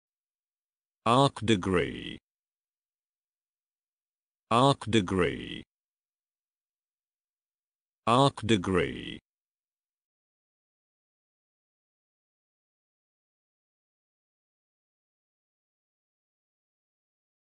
Astronomy Live How To Pronounce Arc Degree
I am Astronomy Live and I welcome you to Astronomy Live In this video we are viewing solar activities with the Coronado PST Hydrogen Alpha Telescope. Warning: Children Always use a Filter and NEVER look directly at the sun.